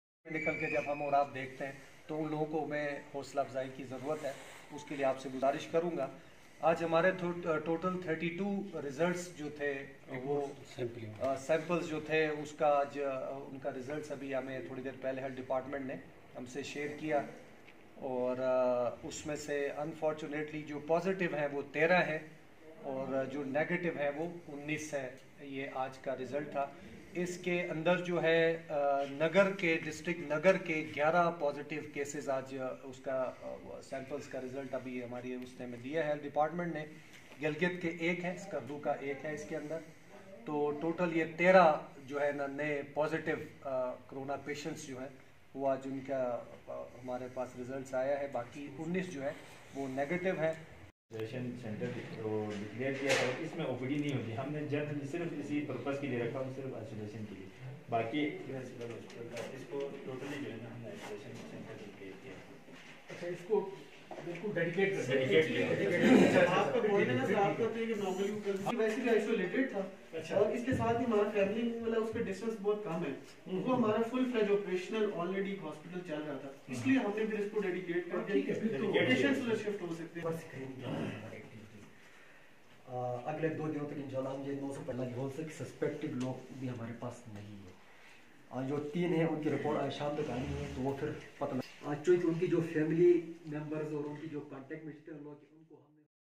گلگت بلتستان میں کرونا وائرس کے مریضوں میں تشویشناک حد تک اضافہ##
Like/subscribes/comments/share